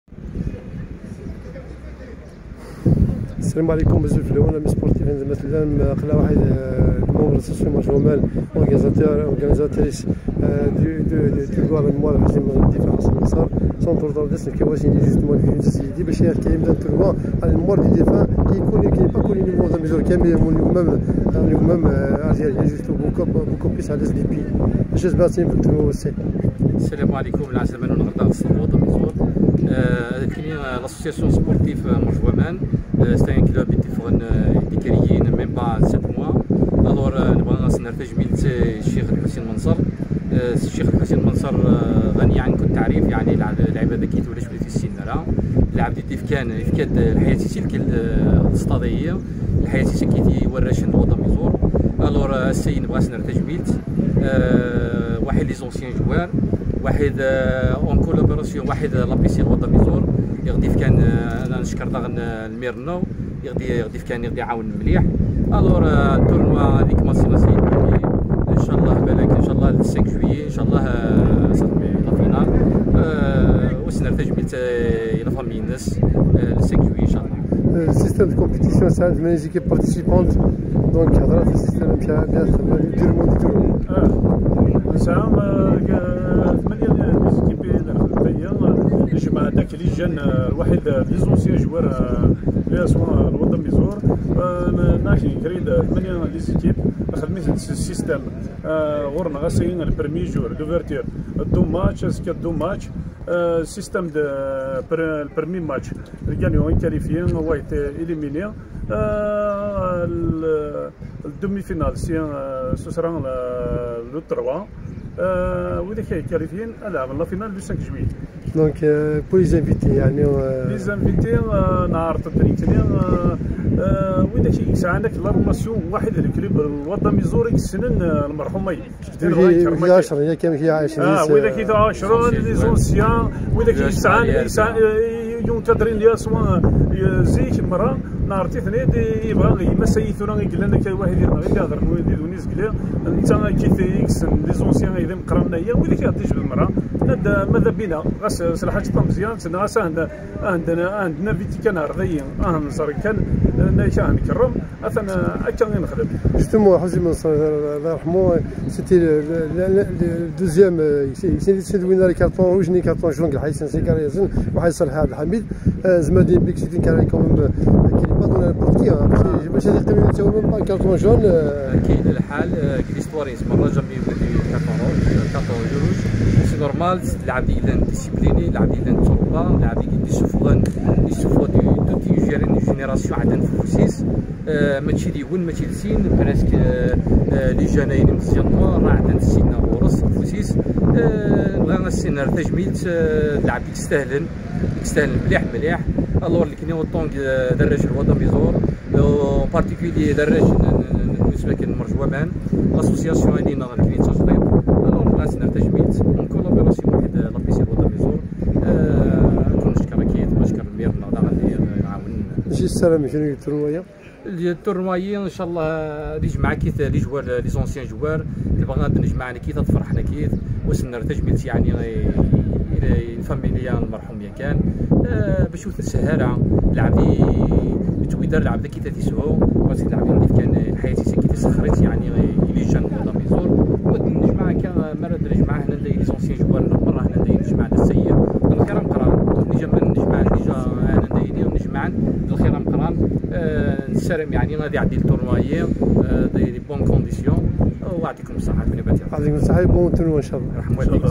Amizour. Tournoi à la mémoire du défunt MANSEUR Hocine. Les organisateurs en parlent.
Amizour. Tournoi à la mémoire du défunt MANSEUR Hocine.

Les témoignages
Les organisateurs (US MERDJ OUAMANE) en parlent.